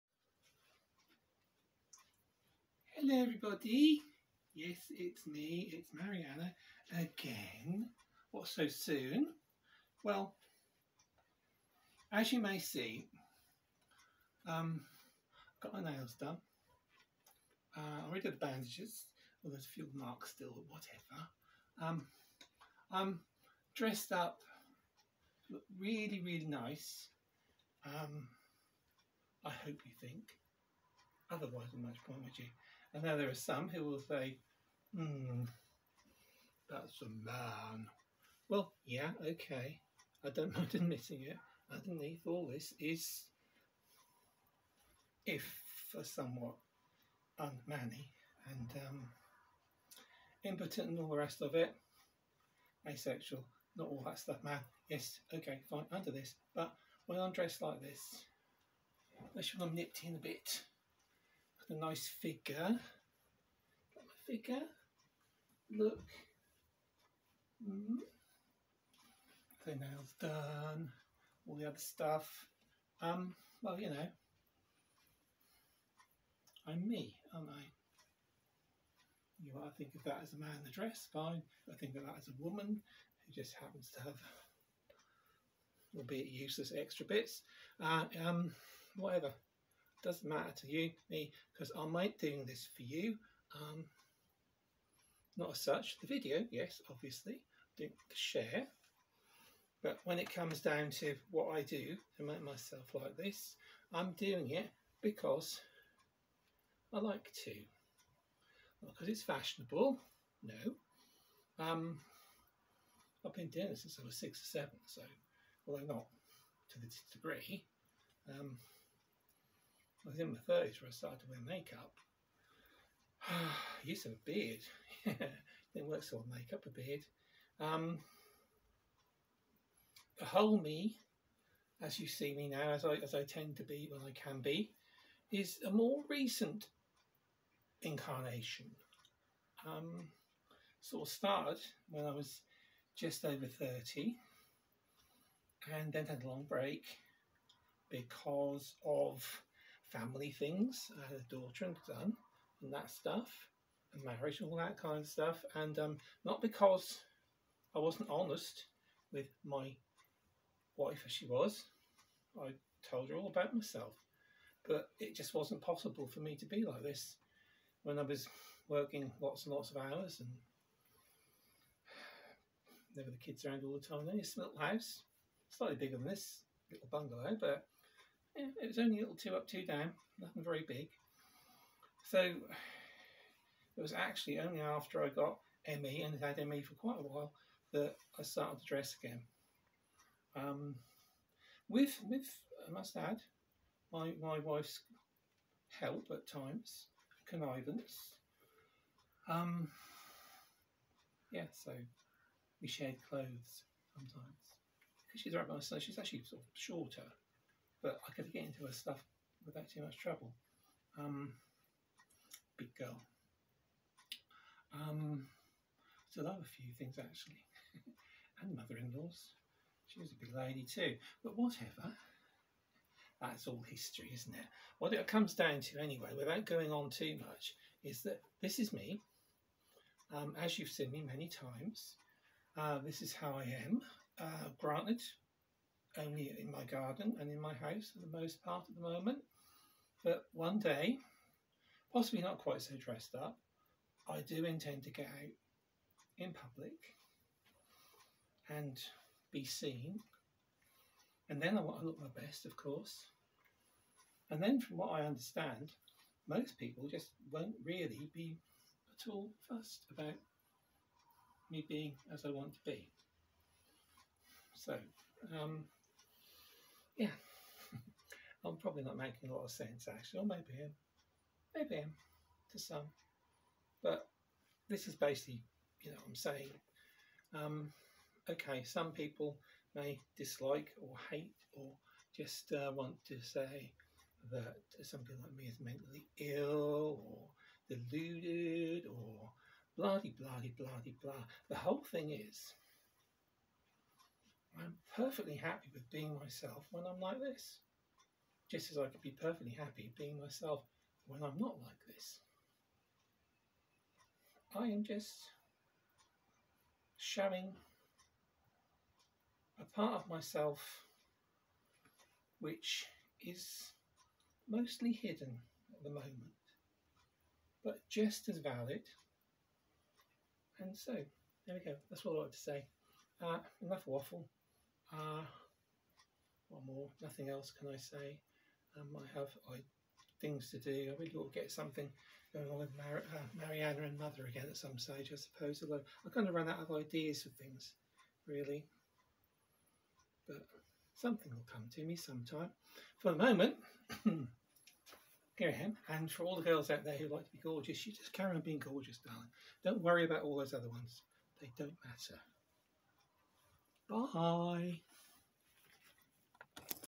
Feeling good. Crossdresser
Probably all stuff I've said before but mostly say I don't care if people do like me being trans, I am what I am and happy with myself. No deluded, not mentally ill ( saying so is an insult to those with genuine mental health issues). Stuff like that.